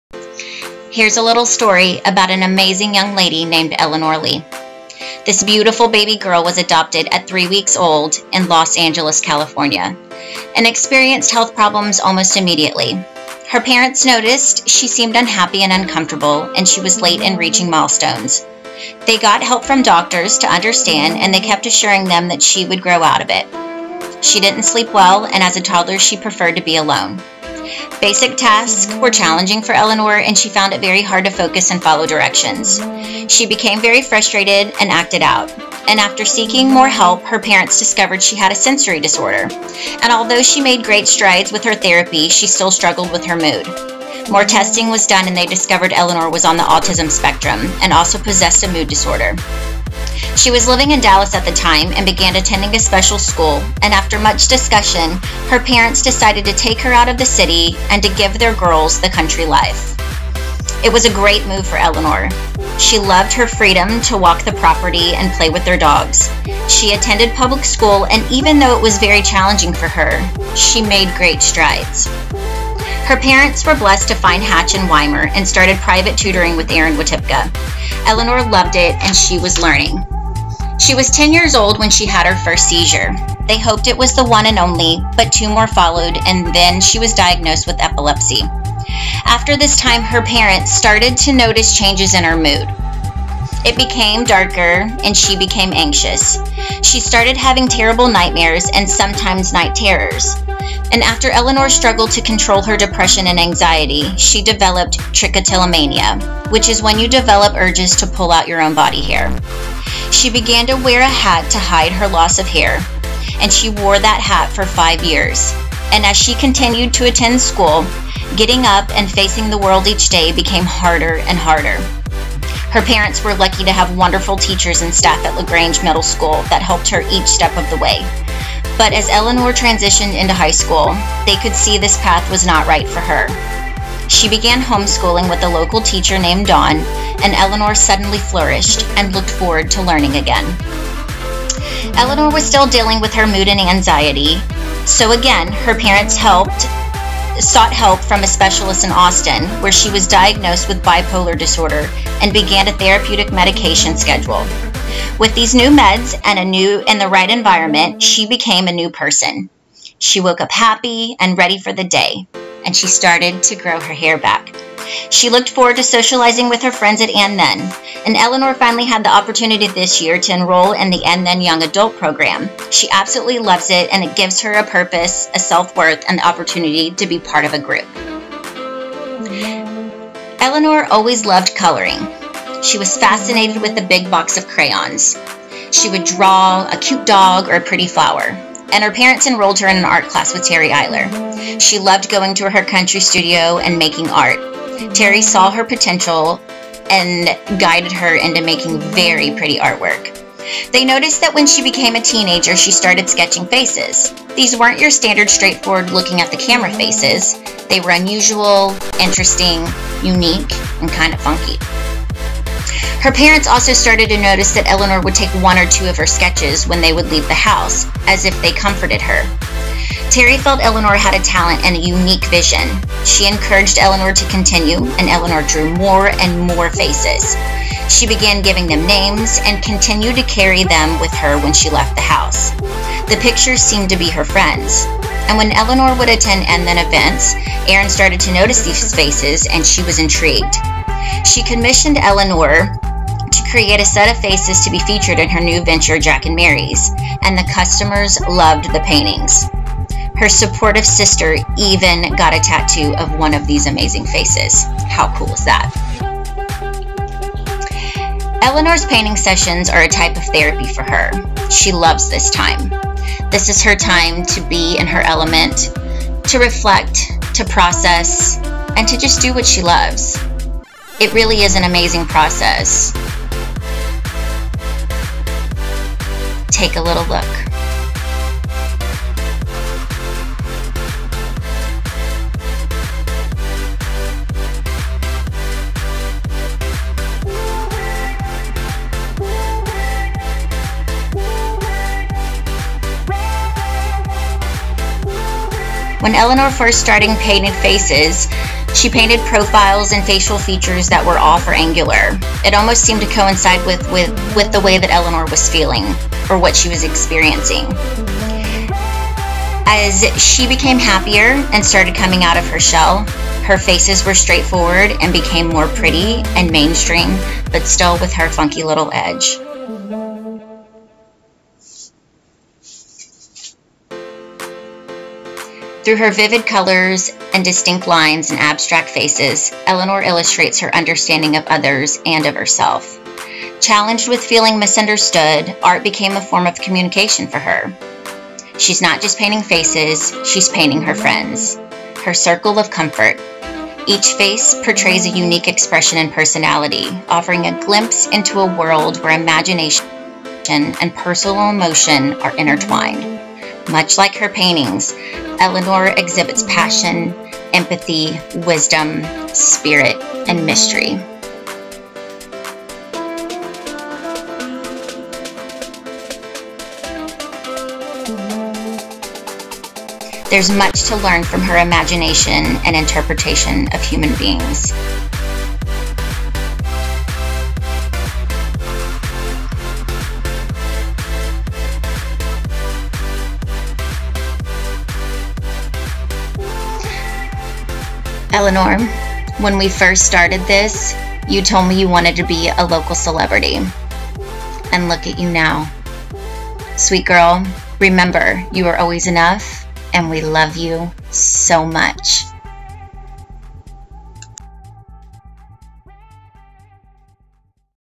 Eleanor Lee | And Then hosts Eleanor Lee's first Art Show | June 19, 2021
Eleanor Lee “Ellie” Benbenek
Eleanor is a young woman whose art mirrors her own complexity. She is autistic, epileptic, and bipolar, but that is not what defines her.
Through vivid colors, distinct lines, and abstract faces, Eleanor illustrates her understanding of others and of herself. Challenged with feeling misunderstood, art became a form of communication for her. She’s not just painting faces, she’s painting her friends, her circle of comfort. Each face portrays a unique expression and personality, offering a glimpse into a world where imagination and personal emotion are intertwined. Much like her paintings, Eleanor exhibits passion, empathy, wisdom, spirit, and mystery.